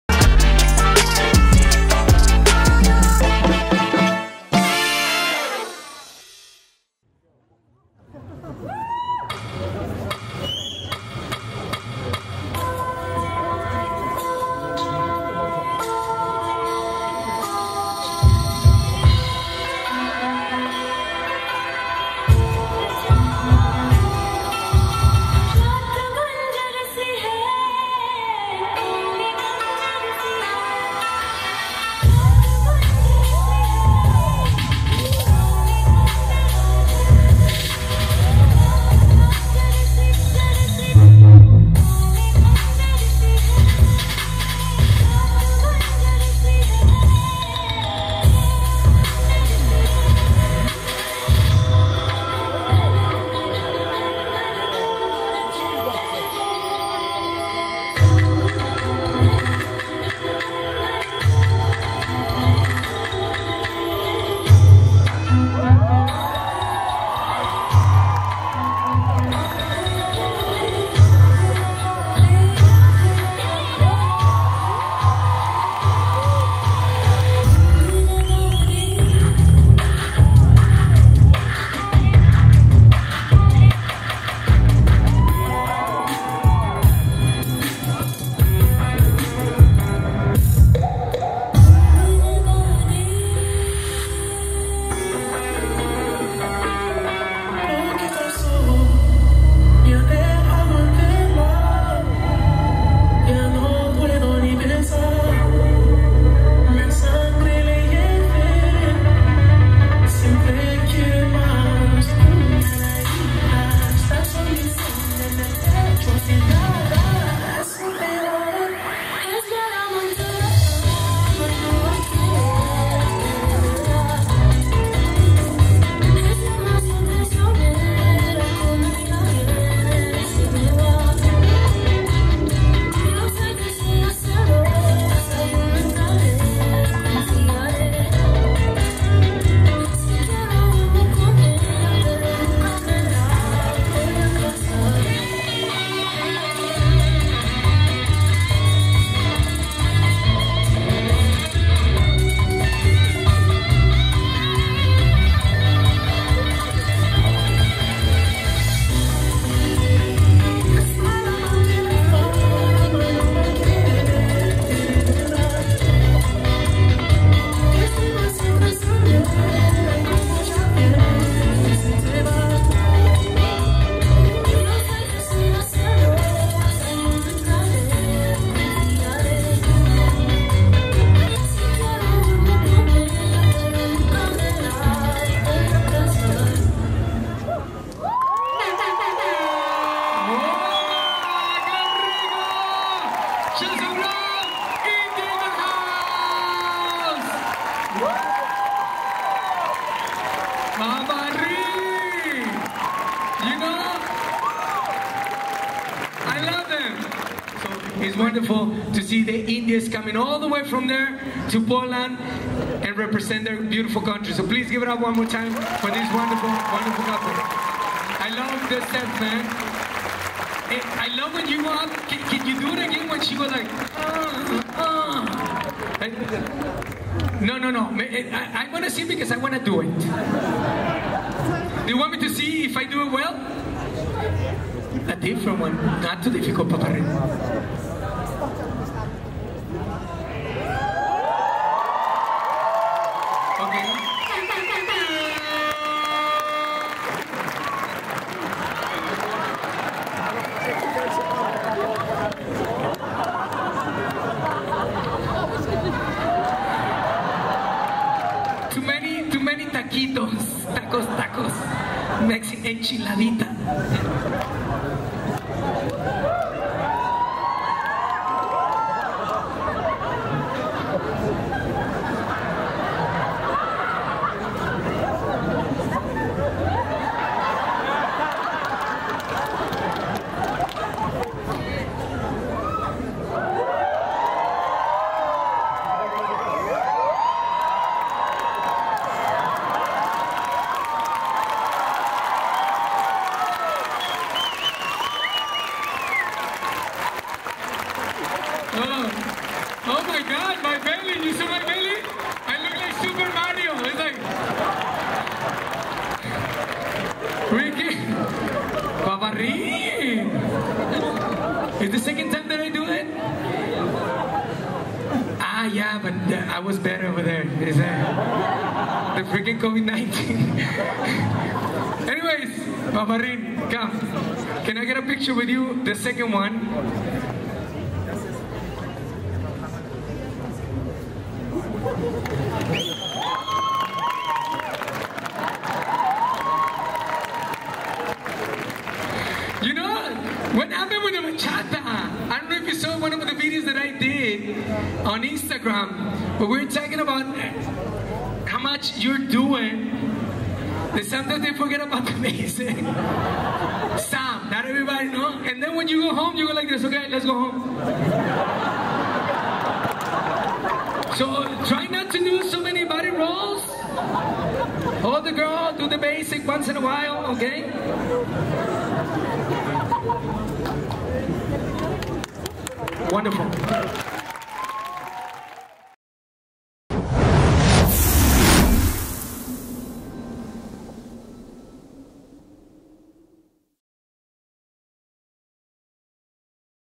Cornel & Rithika show @ Bachaturo 2022
Show by Cornel & Rithika (India) on Friday @. Bachaturo 2022
 🎥 Video by Ricky R Production | Camera operators: Ricky R and Grzegorz Matoga